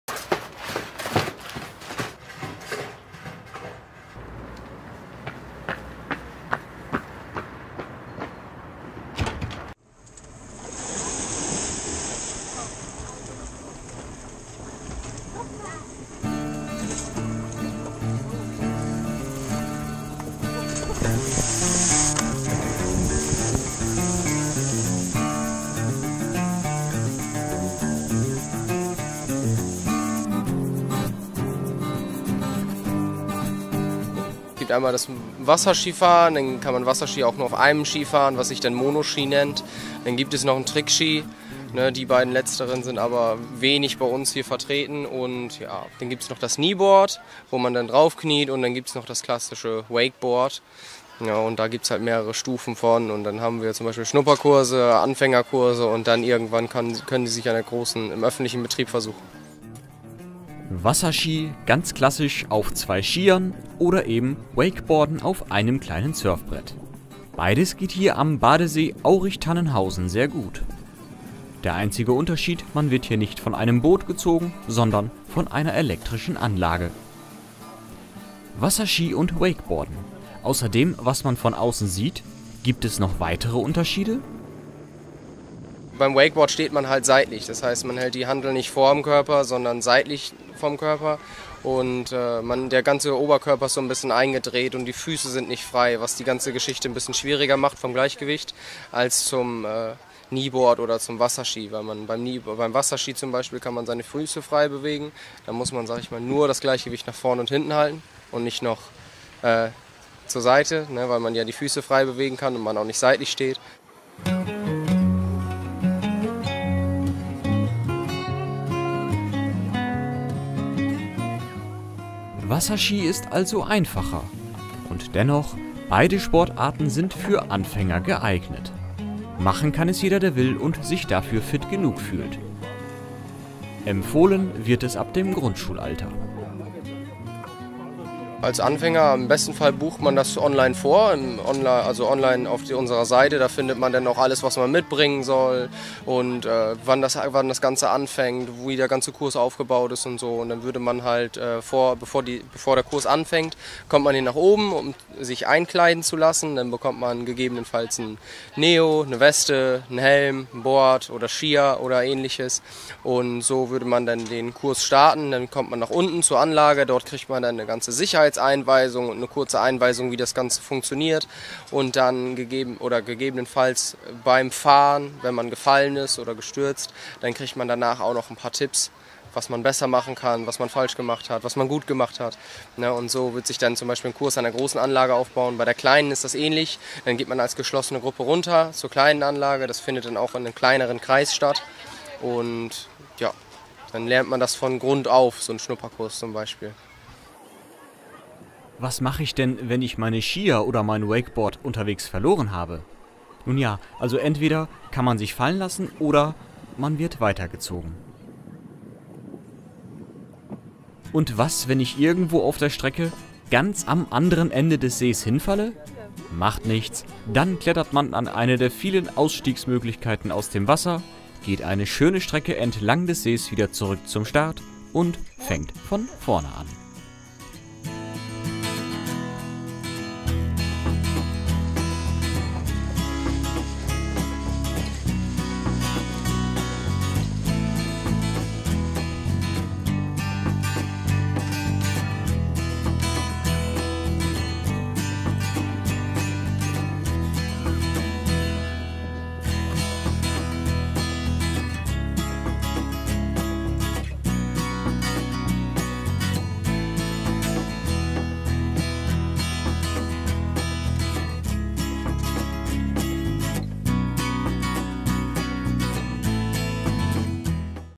Wasserski und Wakeboard in Tannenhausen
Wasserski und Wakeboard kann man beim See in Aurich-Tannenhausen fahren. Auch als Anfänger ist man hier sehr willkommen.